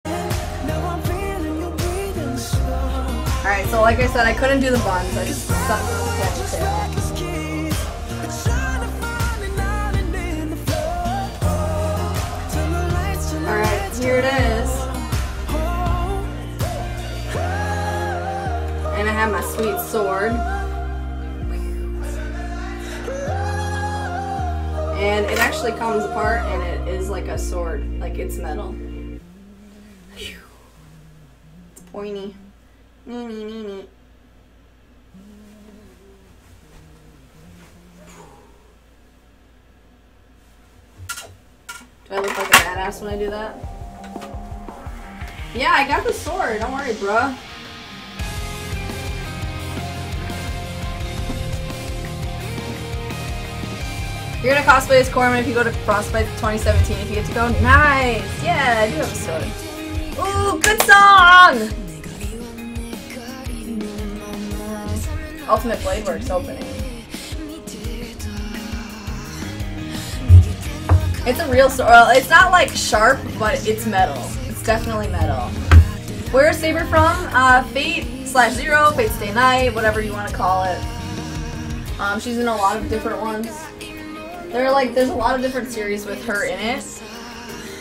SABER COSPLAY!
HB does Saber Cosplay from Fate/Zero.